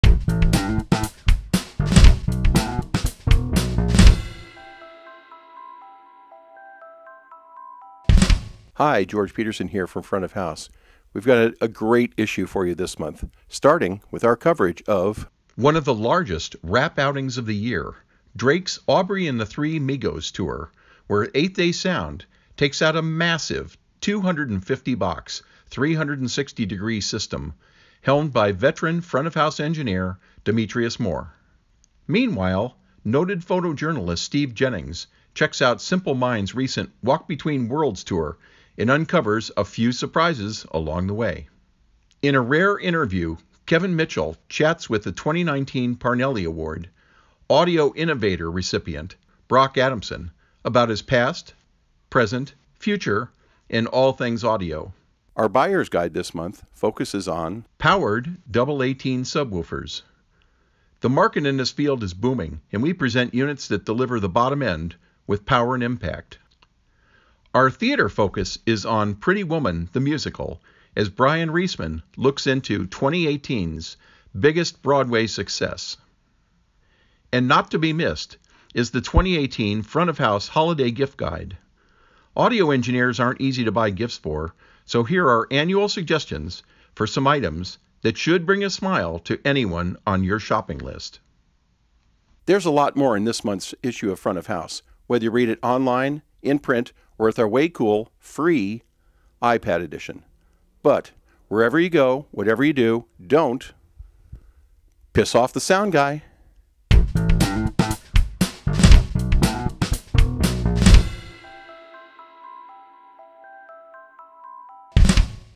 Preview: December 2018 Issue of FRONT of HOUSE
Editor George Petersen previews the December 2018 issue of FOH, which features Drake’s “Aubrey and the Three Migos” tour; Simple Minds’ recent “Walk Between Worlds” tour; our rare interview with the 2019 Parnelli Award Audio Innovator recipient (and Adamson Systems founder) Brock Adamson; a Buyers Guide to
Powered Double-18 Subwoofers; a look into the audio for 2018’s biggest Broadway success “Pretty Woman: The Musical” and the famous annual FOH Holiday Gift Guide, with suggestions for items that should bring a smile to any professional sound engineer  on your shopping list.